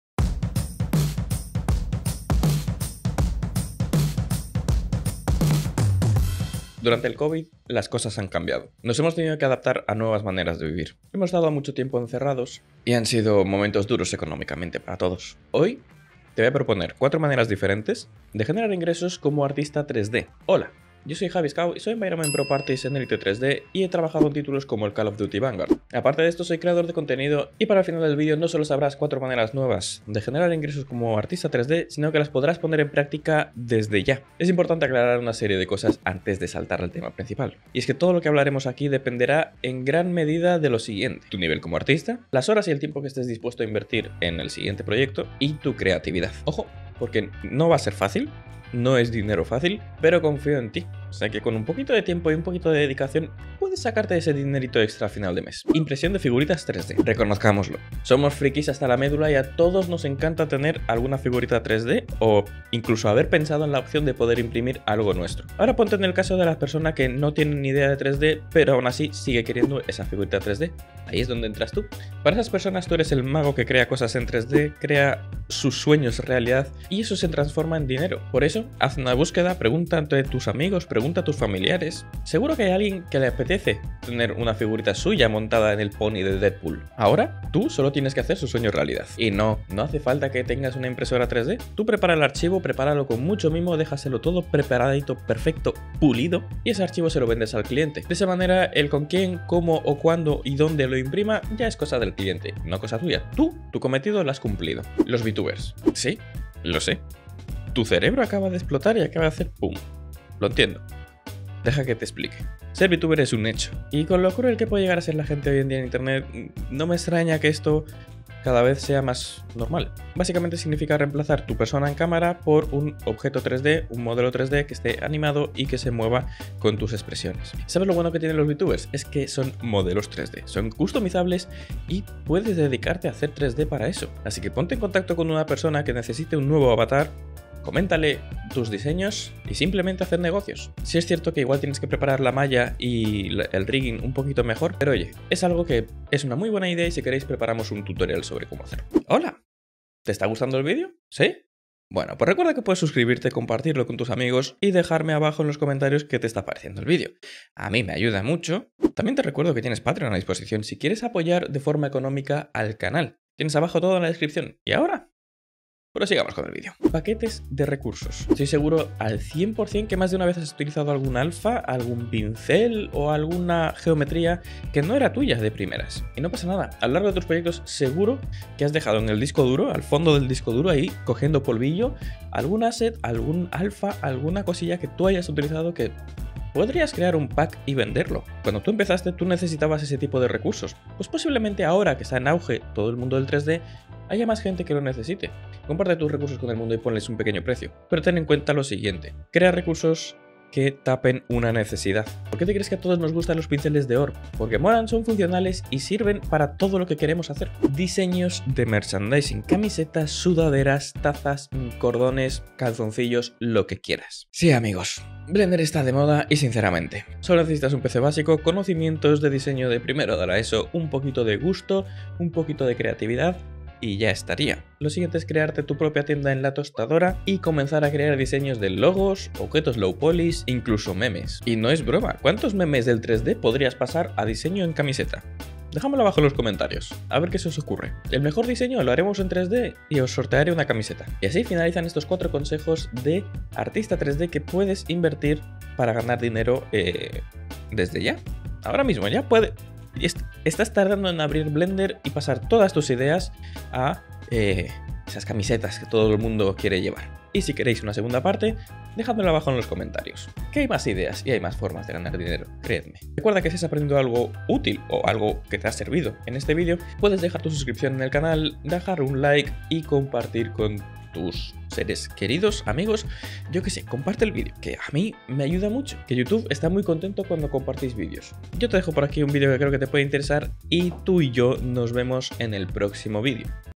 GANAR DINERO HACIENDO 3D
¿Te imaginas vivir del 3D y generar ingresos pasivos? ¡Suena increíble, ¿verdad? En este video, descubrirás cómo puedes convertir tu pasión por el arte 3D en una fuente de ingresos constante y sostenible.

Ser un artista 3D no solo implica crear piezas impresionantes, sino también saber cómo monetizar tu talento. A través de este video, aprenderás diversas estrategias y oportunidades para generar ingresos en línea y transformarlos en ese dinero extra que te permita cubrir el alquiler y mucho más.

Con un poco de paciencia y dedicación, podrás convertir tus habilidades en una fuente de ingresos pasivos. En este video, exploraremos diferentes enfoques, desde la venta de modelos y activos 3D en plataformas especializadas hasta la creación de cursos en línea y la participación en proyectos colaborativos.

Acompáñanos mientras exploramos consejos prácticos y experiencias de éxito de artistas 3D que han logrado convertir su pasión en un estilo de vida rentable. Descubre cómo establecer tarifas justas, promocionar tu trabajo, atraer clientes y construir una sólida reputación en la industria.

Si estás listo para tomar el control de tu carrera como artista 3D y generar ingresos consistentes, este video es para ti. ¡Prepárate para dar un paso más cerca de vivir del 3D y disfrutar de la libertad financiera que siempre has deseado!

🟧 CURSOS UDEMY

🟧 GUMROAD